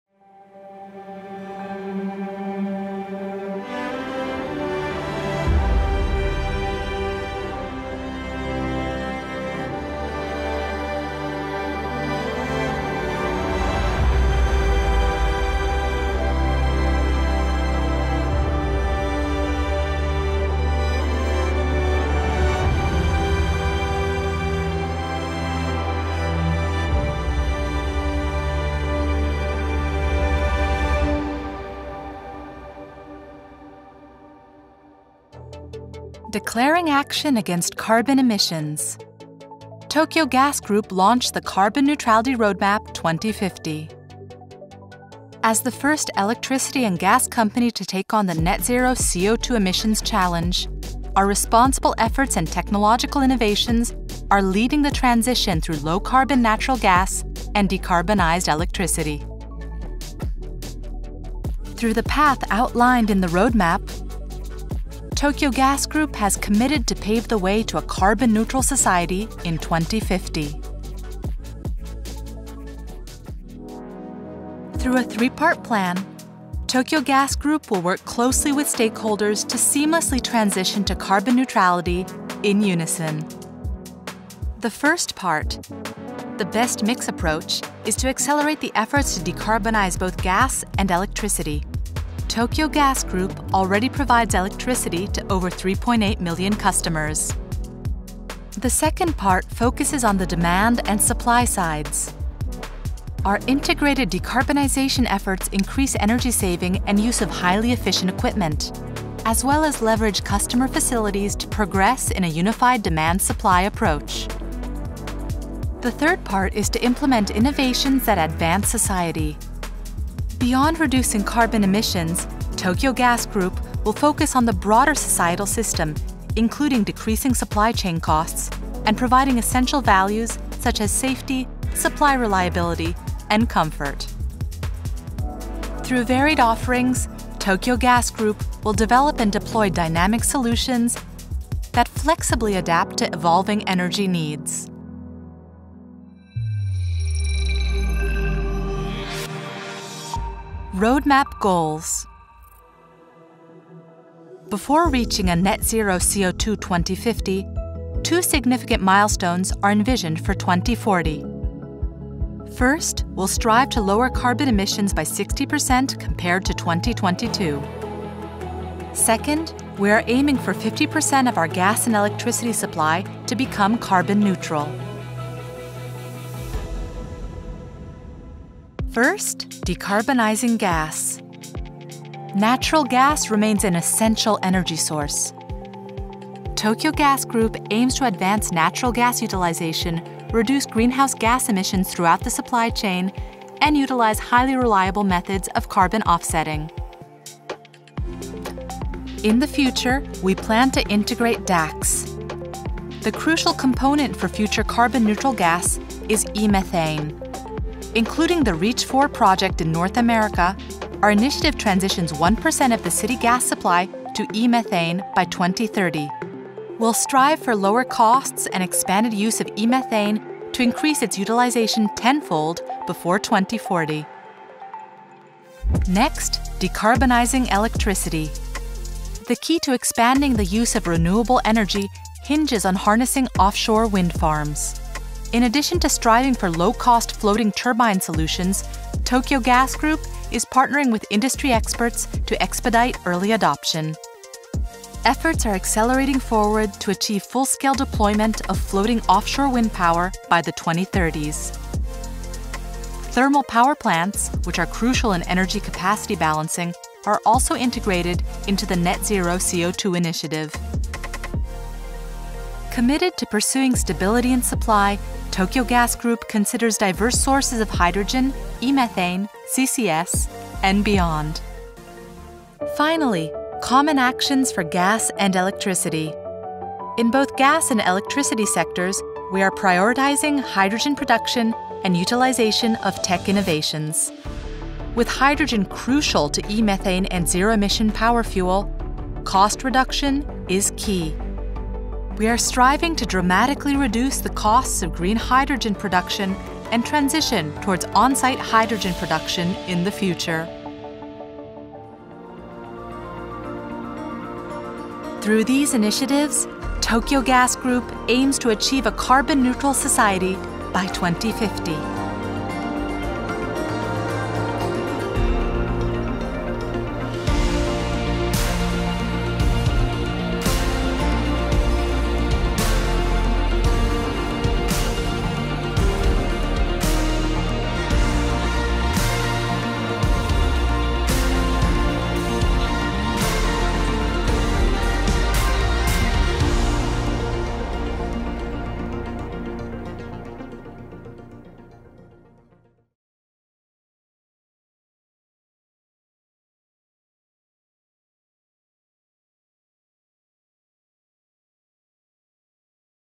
Tokyo Gas Group Carbon Neutrality Roadmap 2050【英語版】
Tokyo Gas Group launched the Carbon Neutrality Roadmap 2050.
This video introduces the “Tokyo Gas Group Carbon Neutrality Roadmap 2050” for your better understanding.